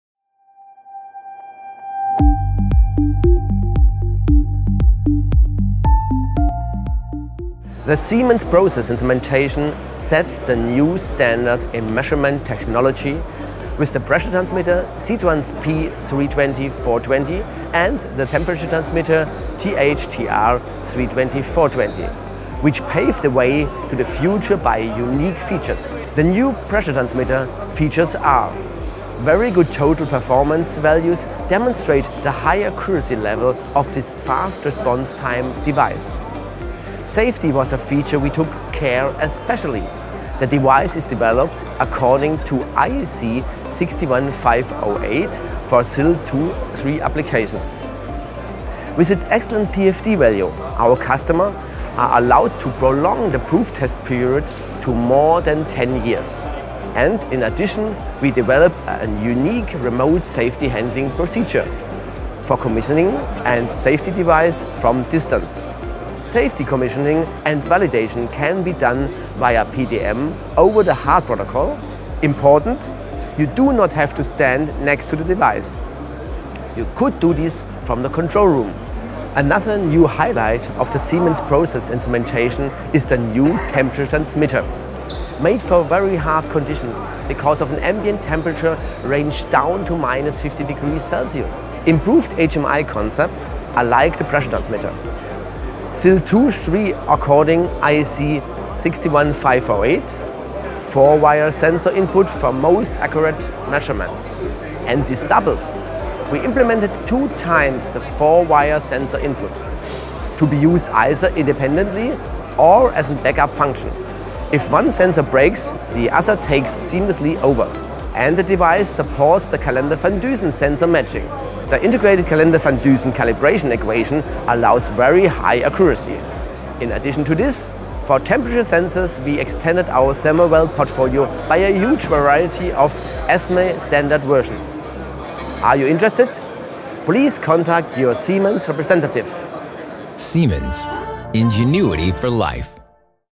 The new generation SITRANS P320/420 and SITRANS TH/TR 320/420
The new generation in SITRANS P and SITRANS T are THE highlights
of process instrumentation at the ACHEMA 2018, which are
characterized by the highest level of technical precision and accuracy.
The new pressure transmitter and the new temperature transmitter pave
the way to digitalization and thus serve as a productivity lever for our
customers.